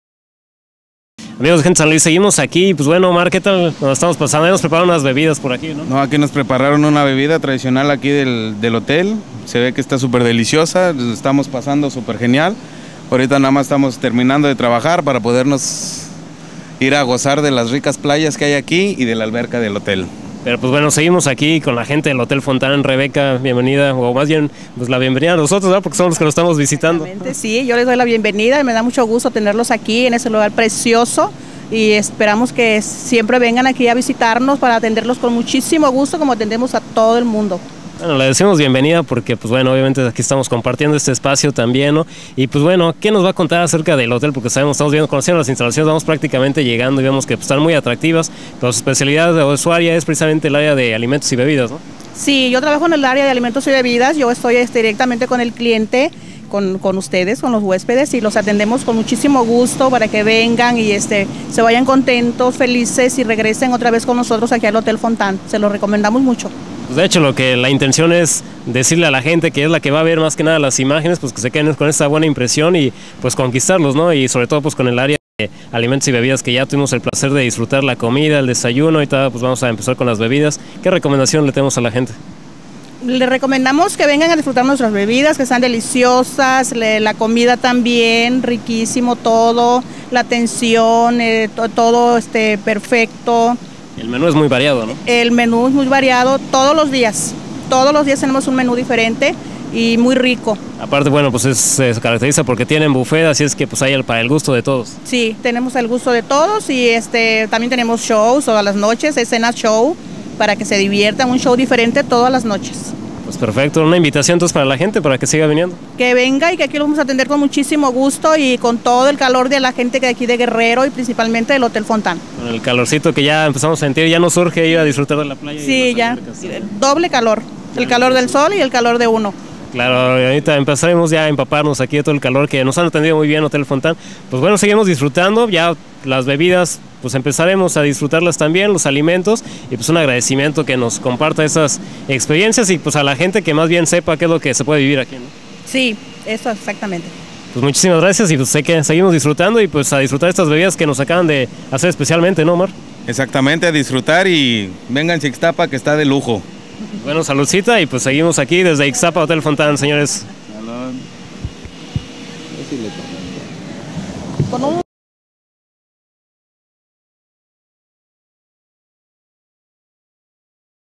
Entrevista a Rebeca de Alimentos y Bebidas Hotel Fontan
No podiamos dejar atras una entrevista con la encargada de alimentos y bebidas del Hotel Fontan para conocer su variedad y servicios que se ofrecen al estar en Ixtapa. siguenos